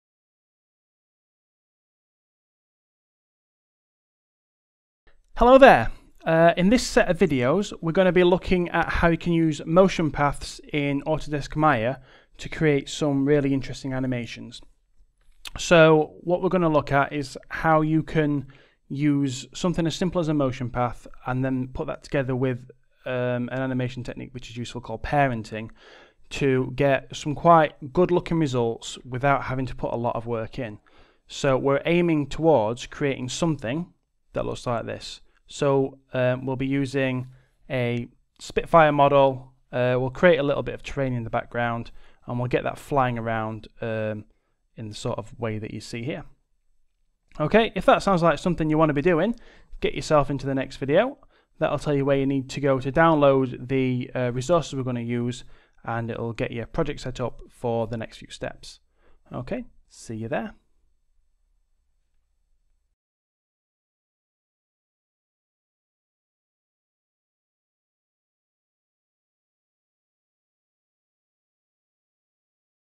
Motion Paths | Maya Animation Tutorial Part 1
This Maya animation tutorial will take you through the process of using motion paths to create a scene like the one in this video. Move on to the next video to begin!

You need Autodesk Maya in order to complete this tutorial. Here's how to get Maya 2016 for free